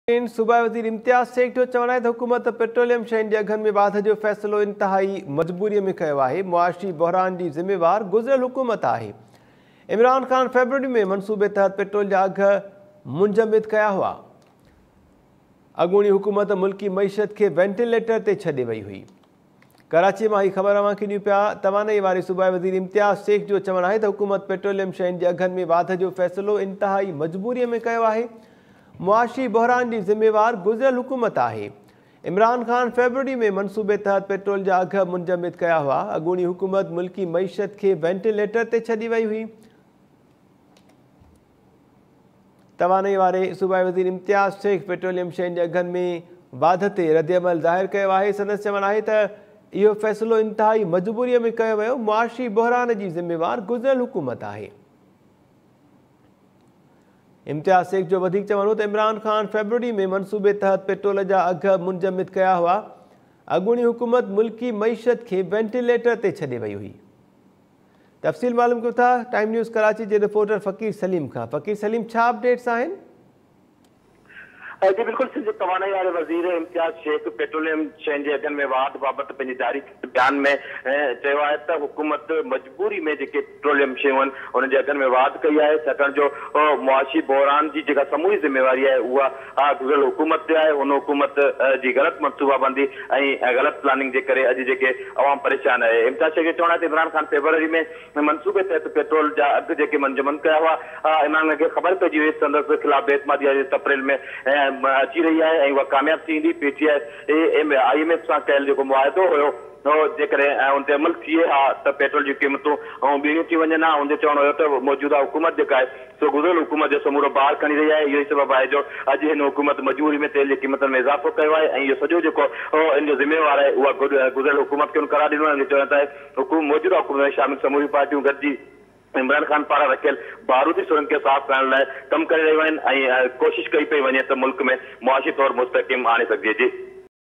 Hukomat Majbori Main Petrol Mahango Kayo Ahy : Imtiaz Ahmed Shaikh
حڪومت مجبوري ۾ پيٽرول مهانگو ڪيو آهي: امتياز شيخ